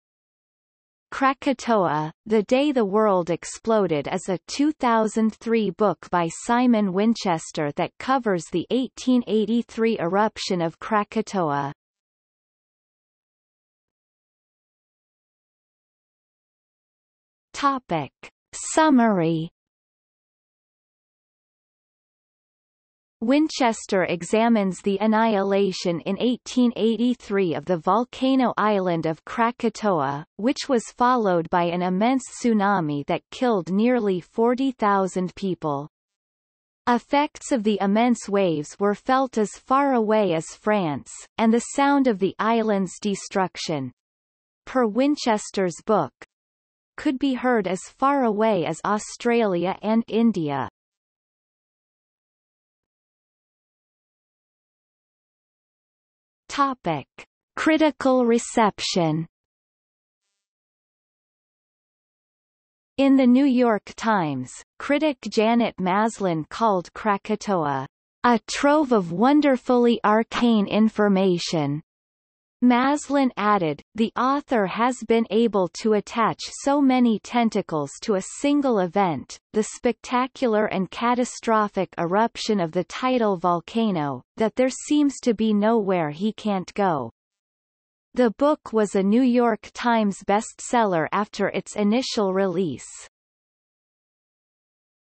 Krakatoa: The Day the World Exploded: August 27, 1883 | Wikipedia audio article
00:00:13 1 Summary
00:00:52 2 Critical reception

- Socrates

SUMMARY
Krakatoa: The Day the World Exploded is a 2003 book by Simon Winchester that covers the 1883 eruption of Krakatoa.